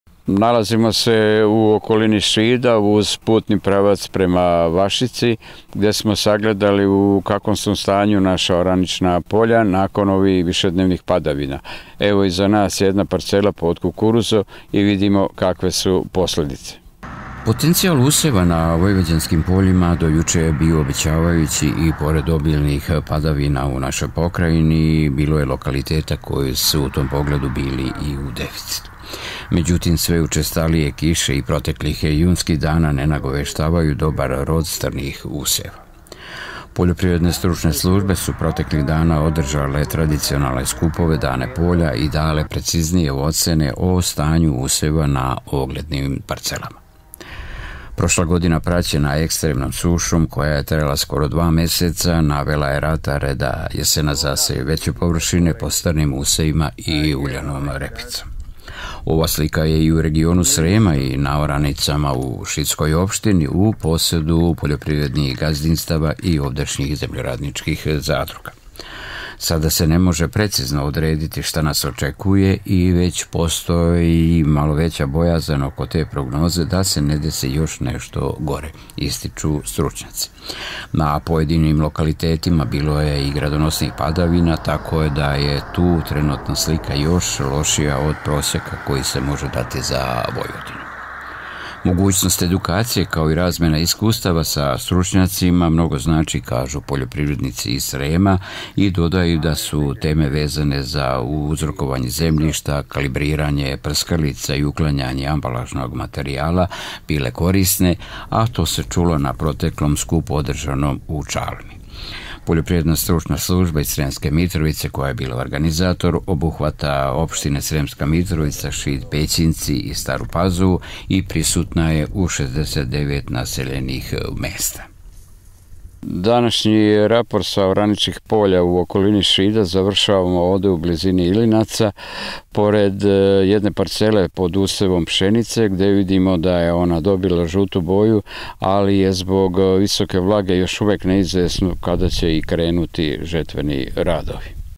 Stanje useva
Prema dostupnim podacima u Srbiji je jesenas strnim usevima zasejano 620.000 hektara, u najvećem delu pšenicom. Protekli period vegetacije praćen je obilnim padavinama i vetrom što je uticalo na stanje useva i očekivan početak sazrevanja i žetve.